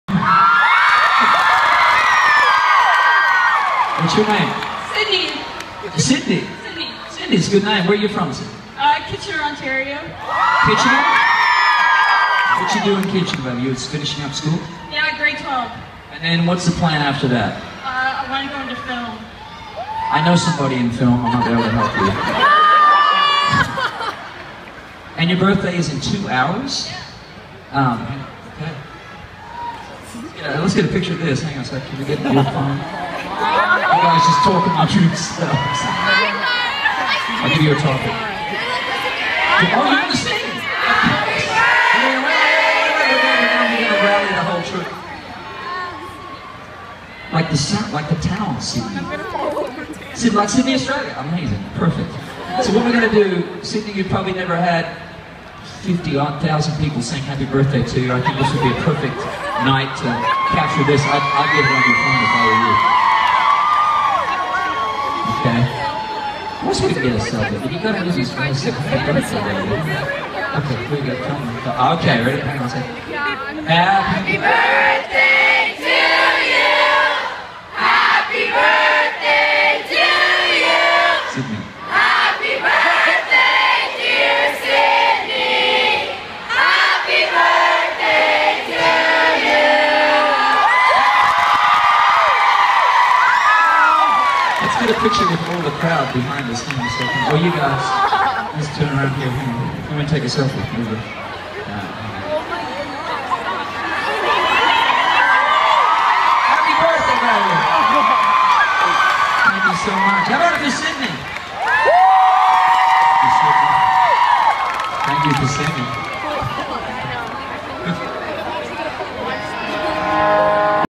Keith Urban London Ontario 2015
Keith Urban giving me the best birthday present I could ever ask for! Truly a dream come true